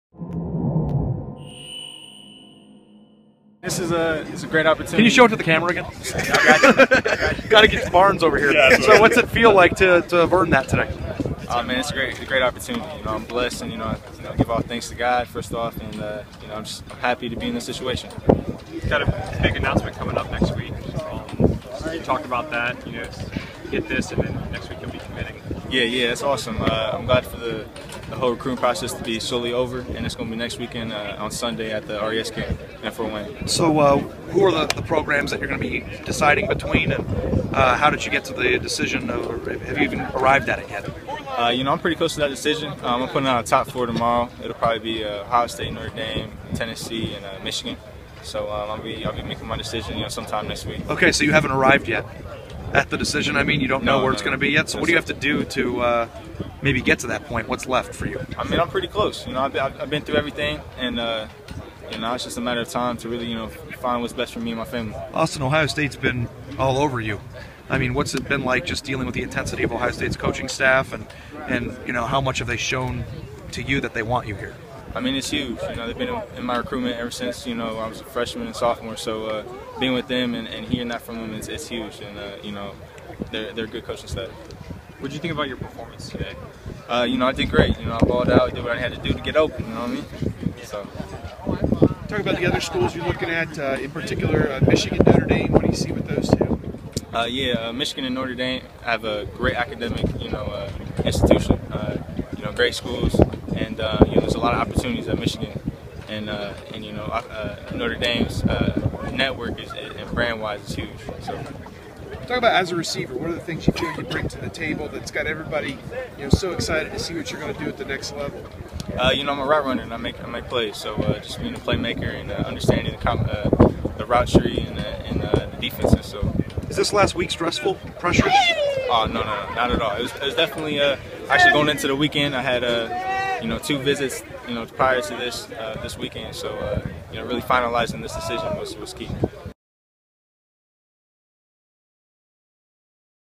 Austin Mack Talks About His Recruitment
Four-star tight end Austin Mack was in Columbus on Sunday for The Opening regional and he earned an invitation. He also took time to talk about his recruitment.